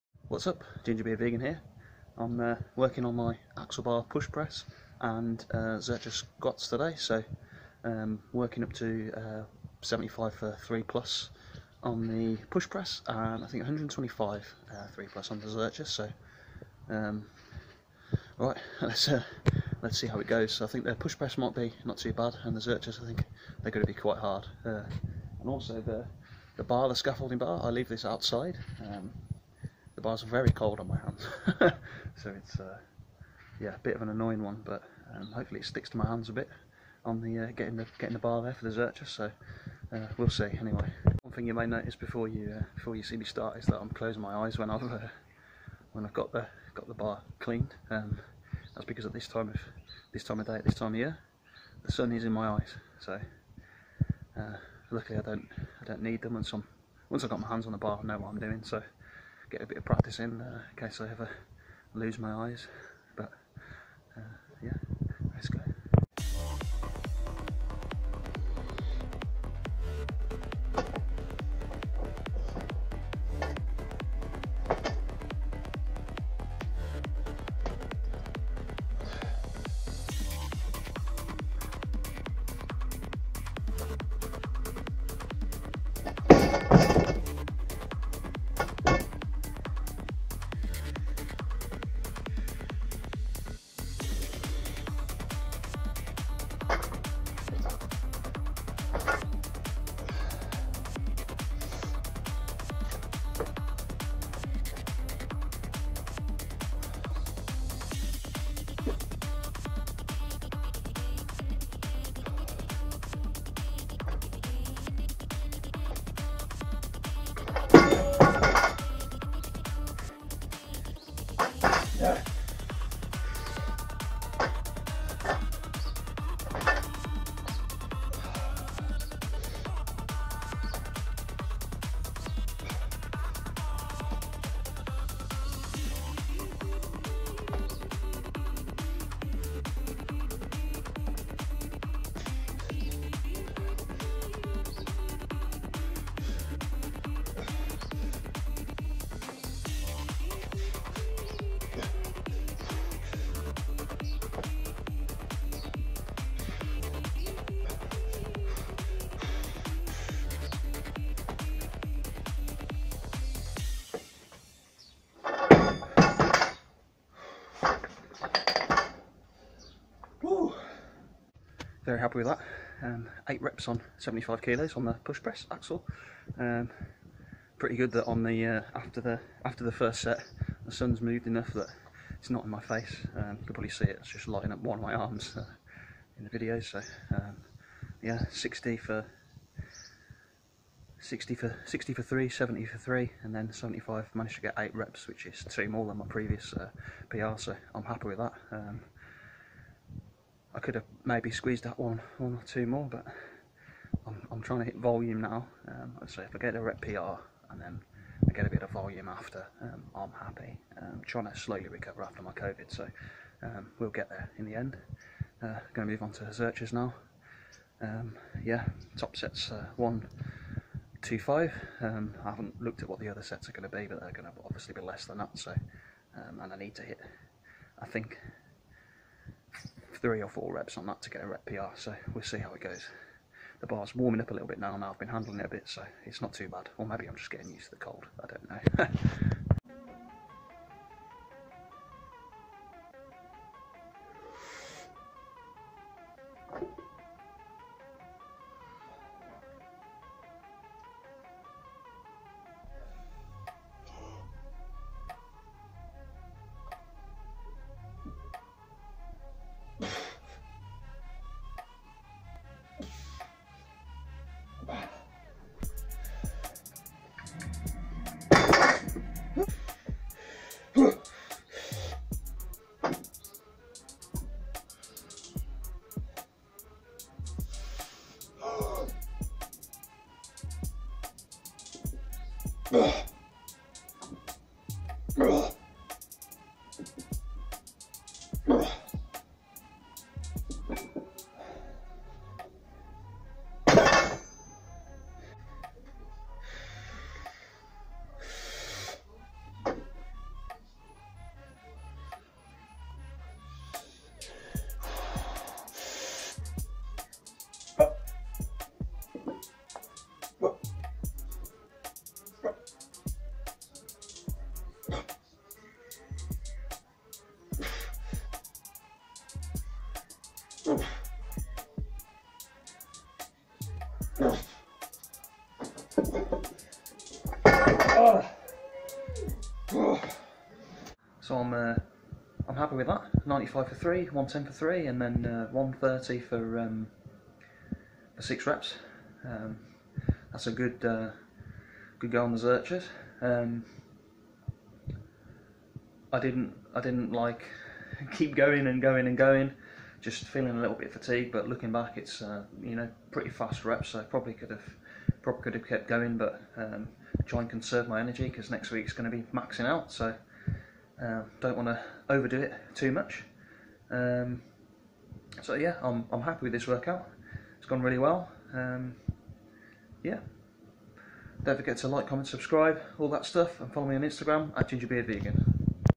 Locked down again! Week 3 Workout 3 | Axle Push Press & Zercher Squats | Vegan Strength
Axle Bar Push Press and Zercher Squats in this workout for 3+ reps. Have a little trouble to start with due to having the sun in my eyes!